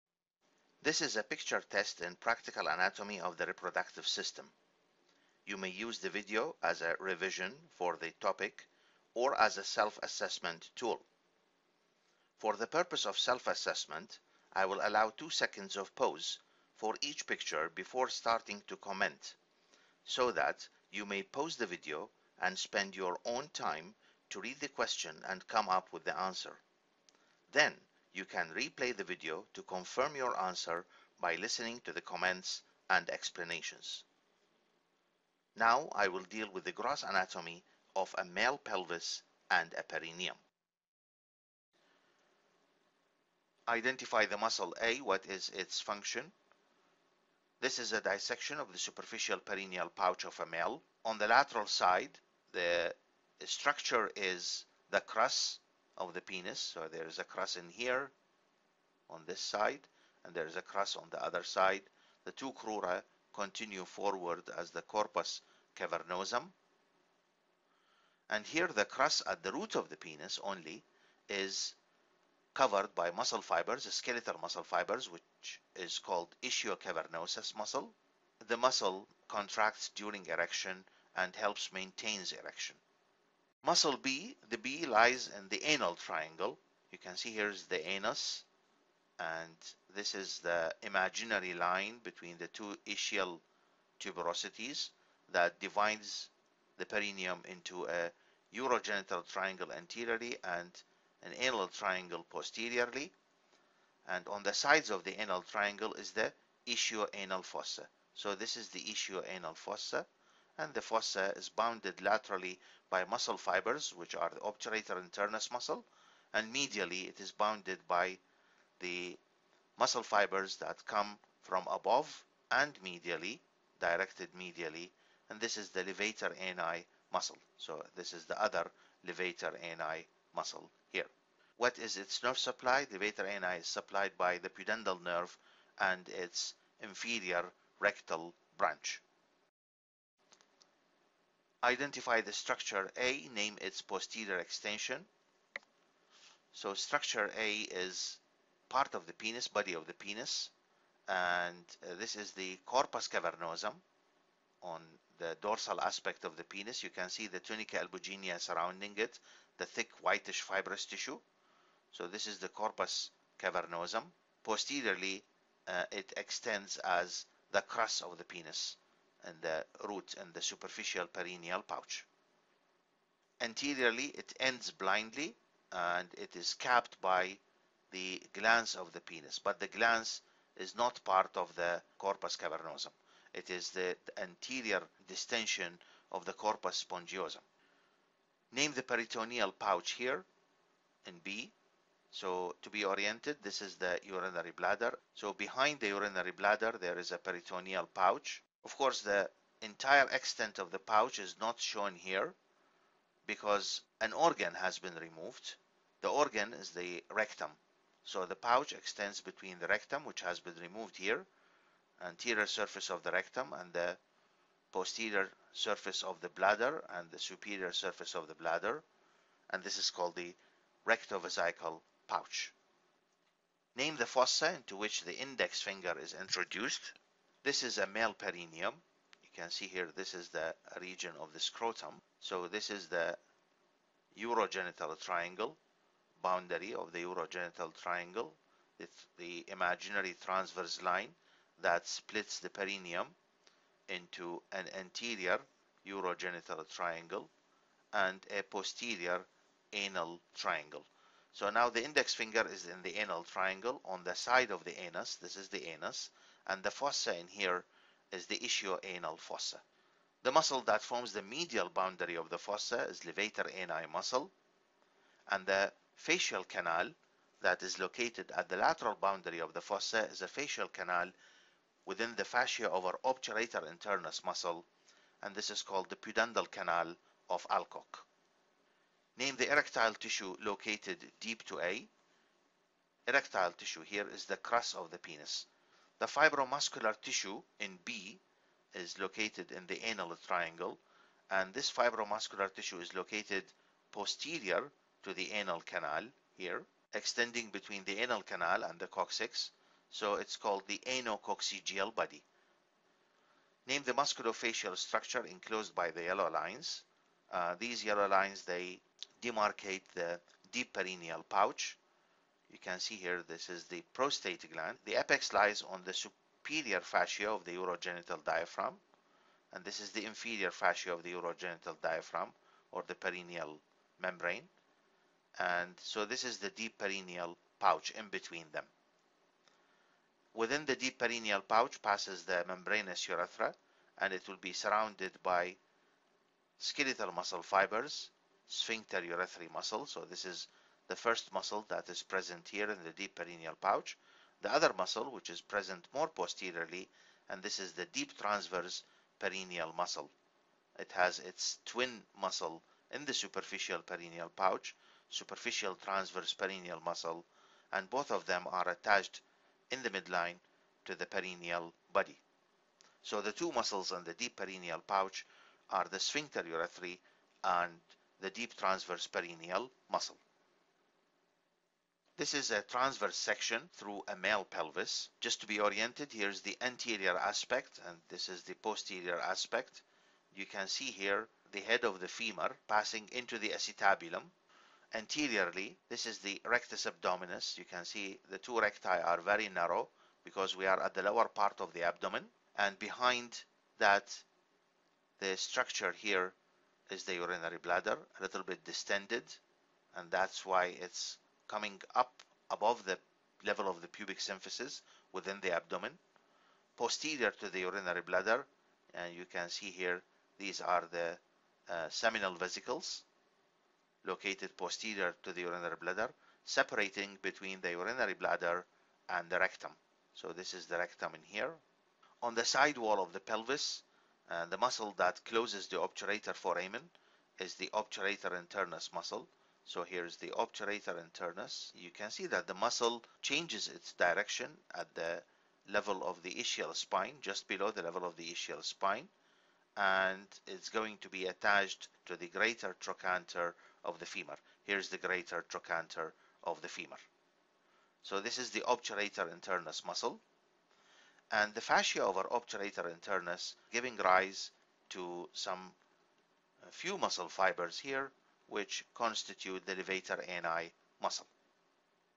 Picture tests in the anatomy of the male reproductive system 3
After completion of this video you will be able to identify some features of the male reproductive system in the pelvis and perineum